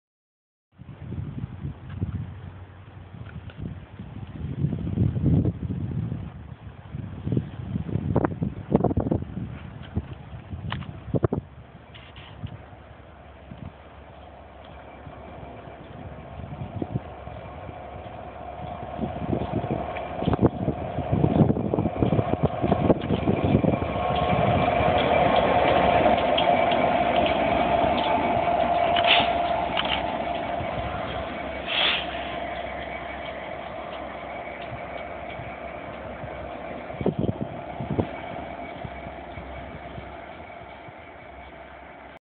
67026 Diamond Jubilee 8 6 13 Walsall
67026 Diamond Jubilee passes Walsall light engine on Saturday 8th June 2013